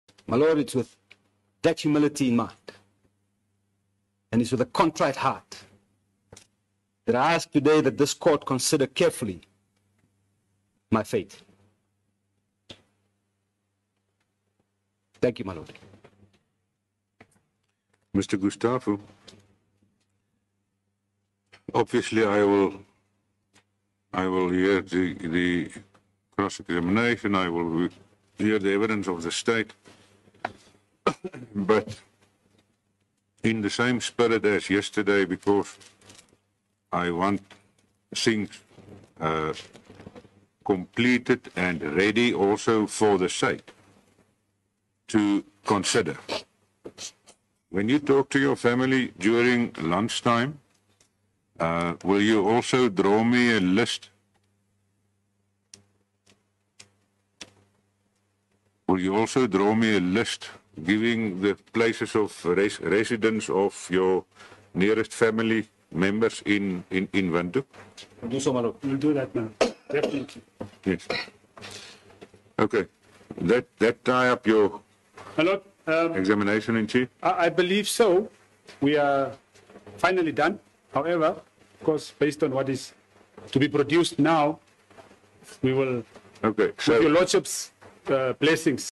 Ricardo Gustavo's testimony ends in tears
Fishrot case accused Ricardo Gustavo closed off the first part of his testimony in support of an application to be granted bail with an emotional address in the Windhoek High Court today. Gustavo has been held in custody for nearly two years.

Camera:Laimi Fillemon
Video edited by:Laimi Fillemon